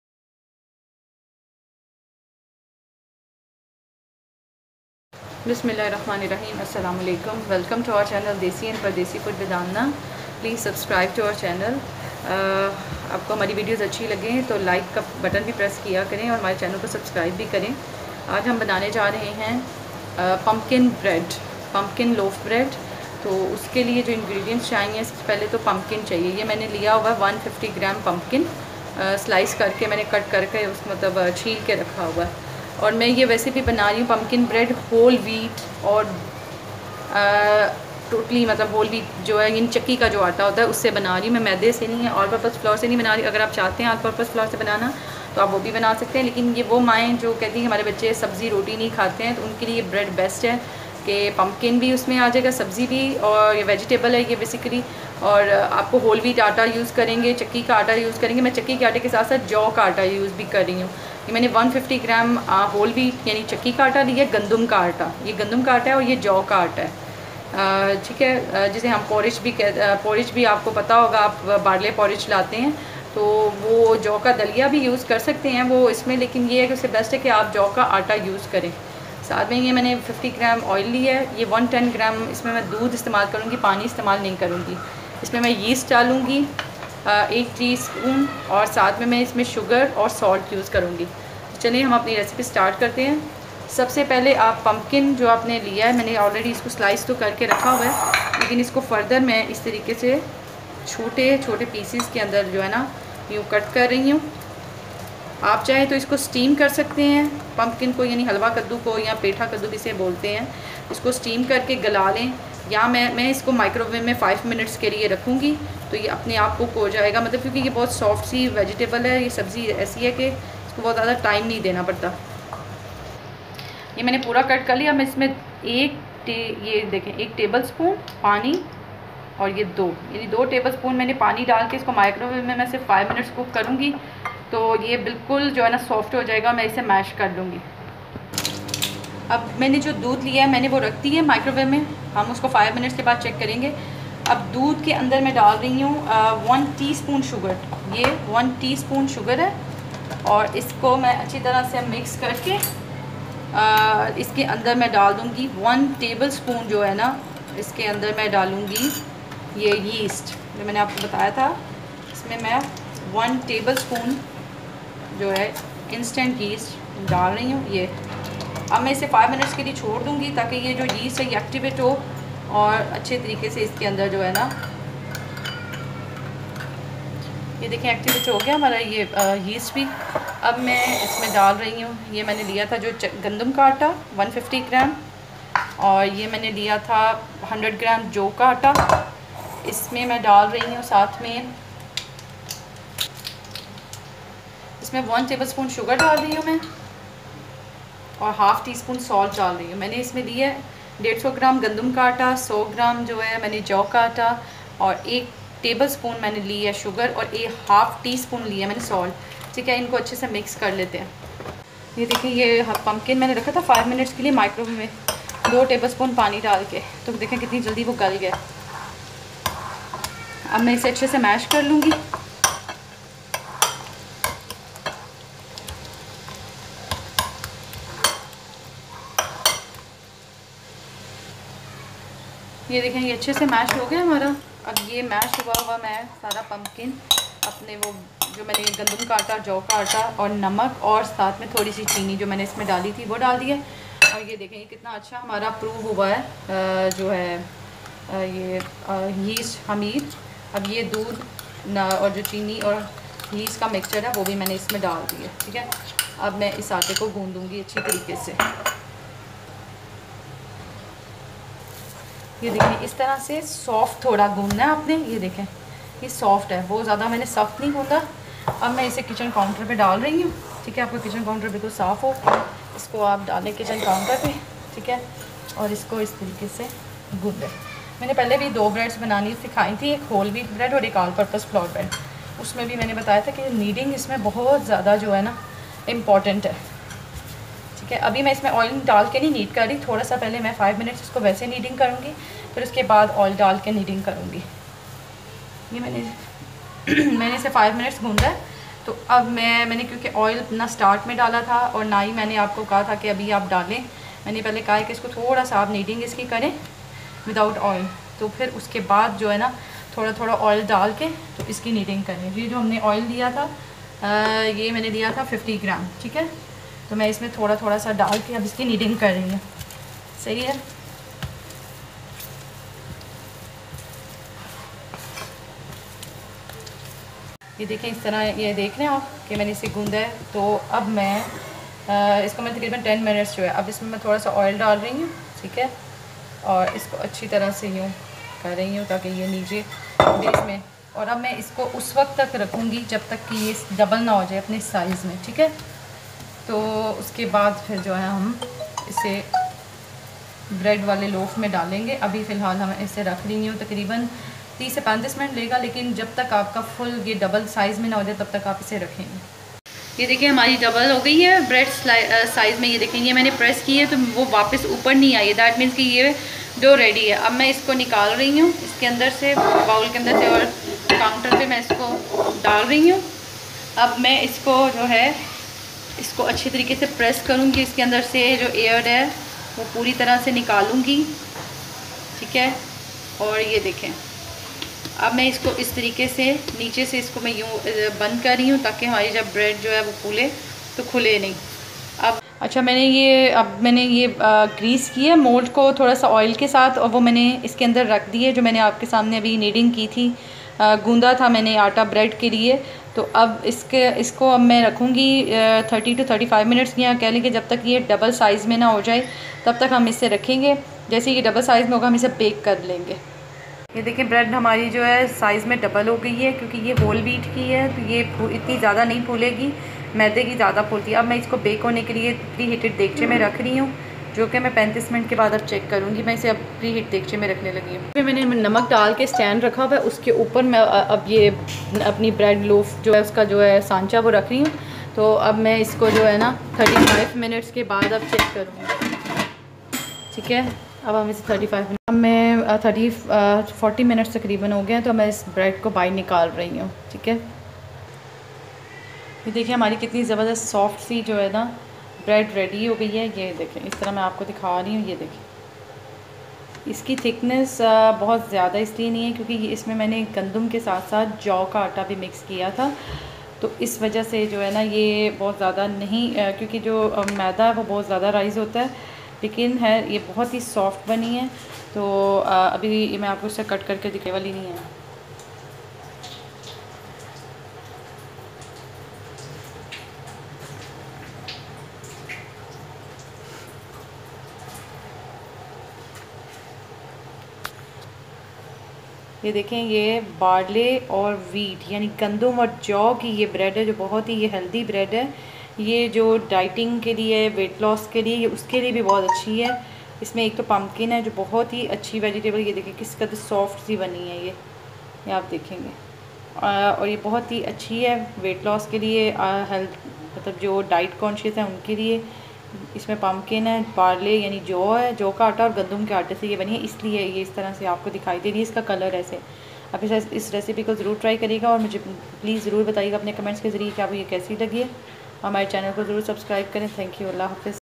Home baked soft pumpkin wheat barley bread without oven
This is a very healthy bread for kids as well as adults.It is also used in dieting and for weight loss purpose.I have used whole wheat and barley in it.